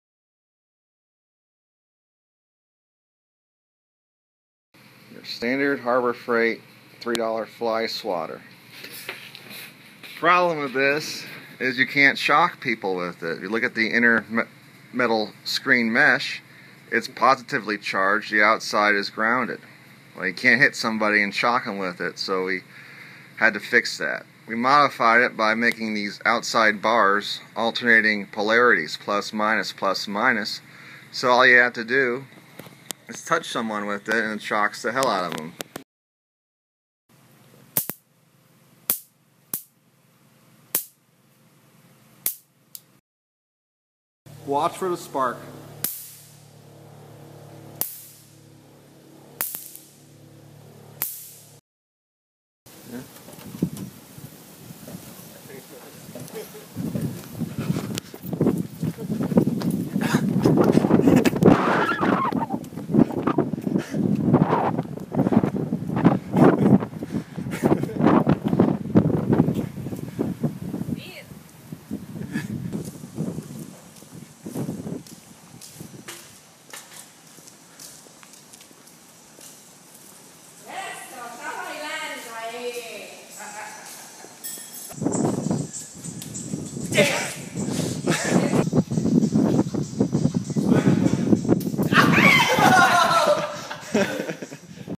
People Zapper
Modified Harbor Freight electric fly swatter so it can shock people on contact!